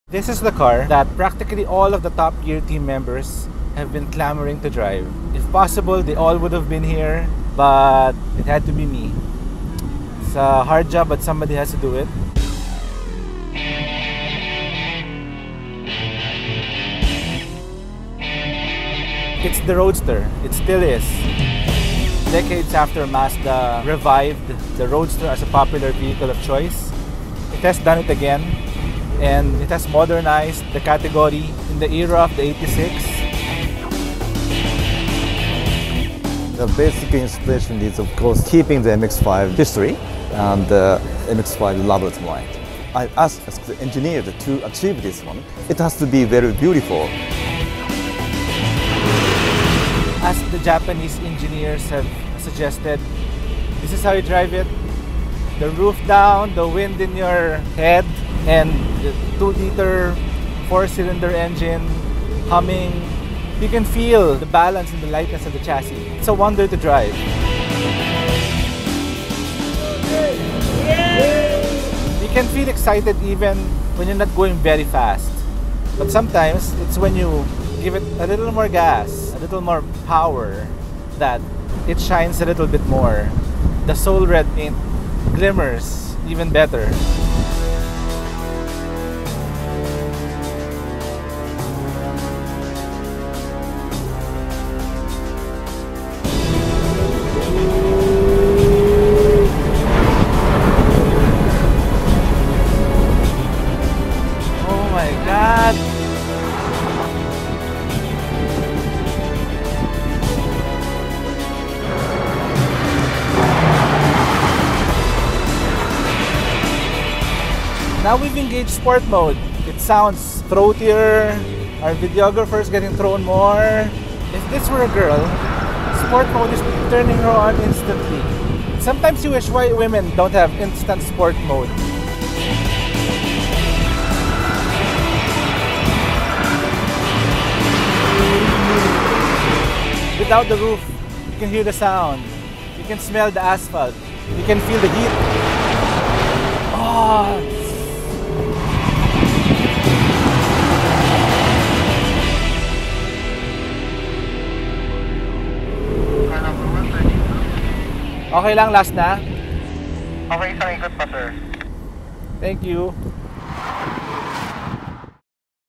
Mazda MX-5
Without a doubt, the Mazda MX-5 belongs to (and surely deserves) an open road where it can let loose and be free to roam. In this Top Gear video, we travel south with the Mazda Philippines crew to Batangas Racing Circuit. Is the roadster equally as fun on a winding track? Click play to find out.

Dig cars?